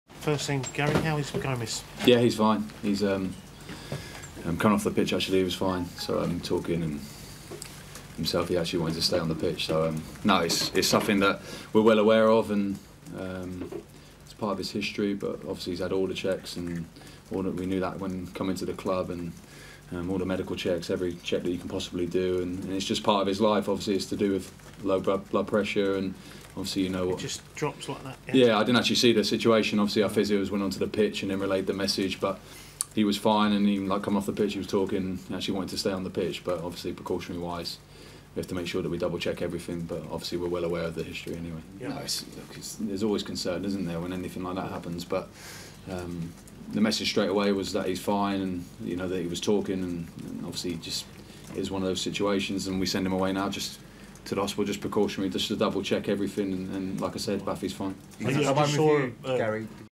VIDEO: 'Bafétimbi Gomis is fine' Swansea boss Monk
Bafetimbi Gomis wanted to carry on playing against Tottenham Hotspur despite fainting on the pitch in their 3-2 defeat, Swansea boss Garry Monk has revealed.